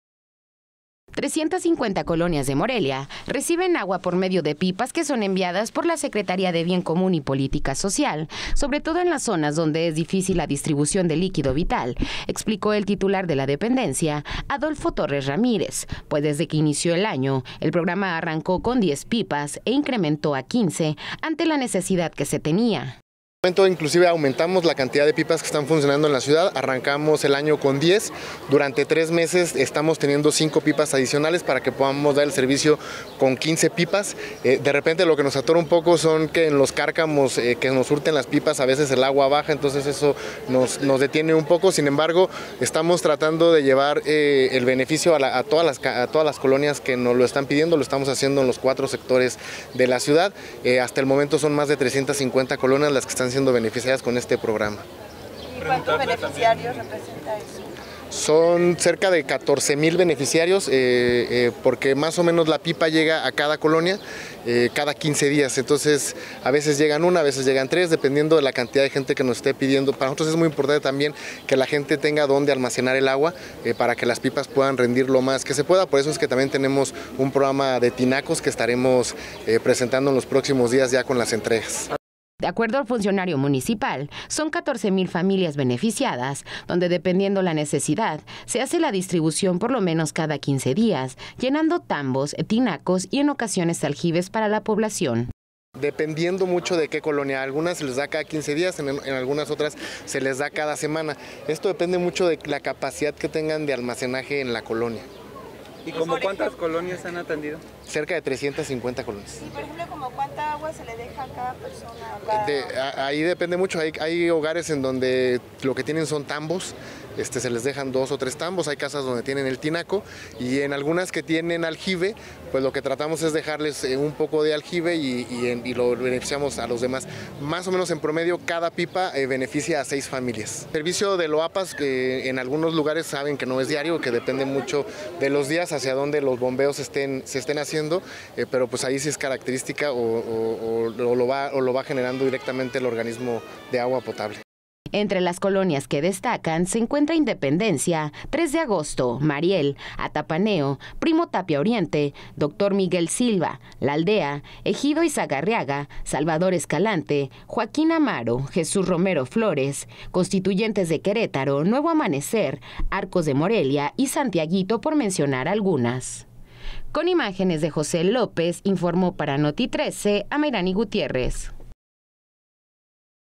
Colonias de Morelia reciben agua a través de pipas por la falta de esta.
Local 🔴 La situación de falta de agua en la ciudad es complicada hoy día son 350 colonias de Morelia que reciben agua por medio de pipas.